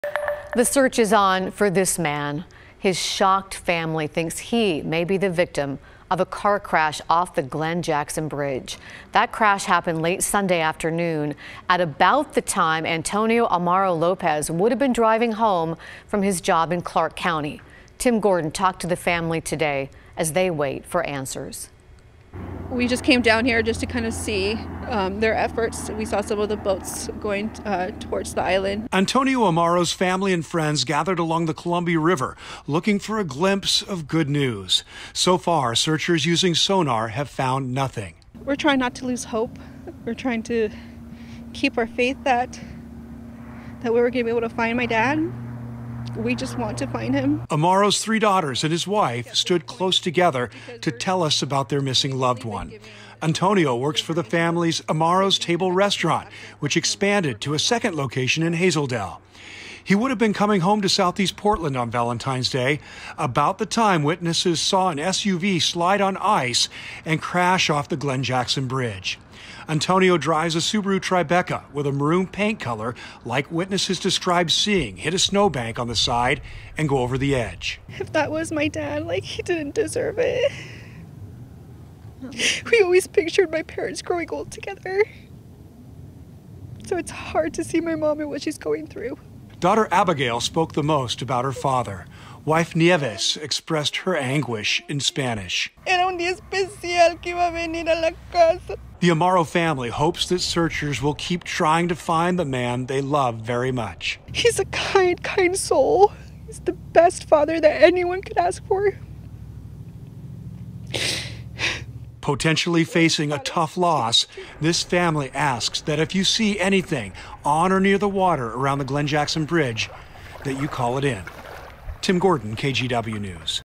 Family fears loved one crashed off I-205 bridge
Authorities are searching for a man whose shocked family thinks may be the victim of a car crash off the Glenn Jackson Bridge, which serves as Interstate 205 and connects Portland to Vancouver.

The crash happened at about the time Antonio Amaro Lopez would have been driving home from his job in Clark County.